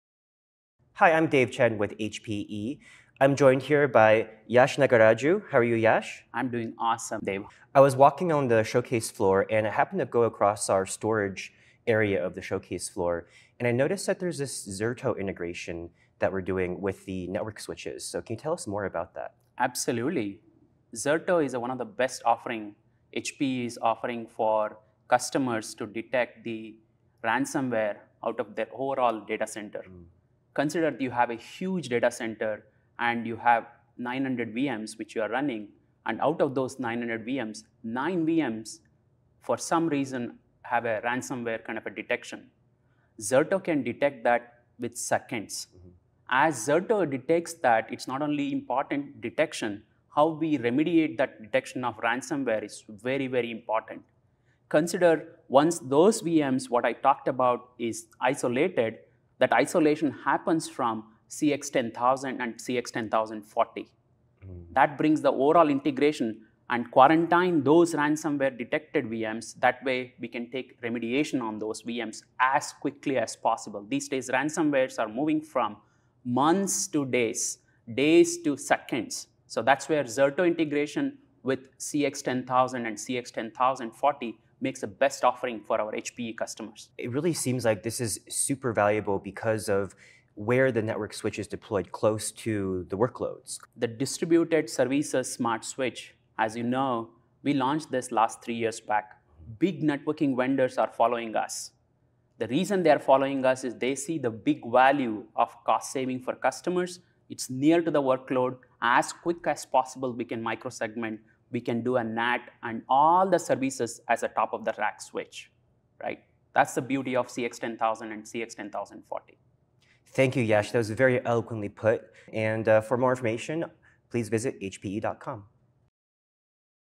Technical Director shares details on CX switch integration with Zerto
Yash Nagaraju, technical director at HPE Aruba Networking is at HPE Discover 2025 to highlight the value of AI for networks and networks for AI. In this clip, he discusses the unique value of using HPE Aruba Networking CX switches alongside Zerto deployments to enhance ransomware response and network-based remediation.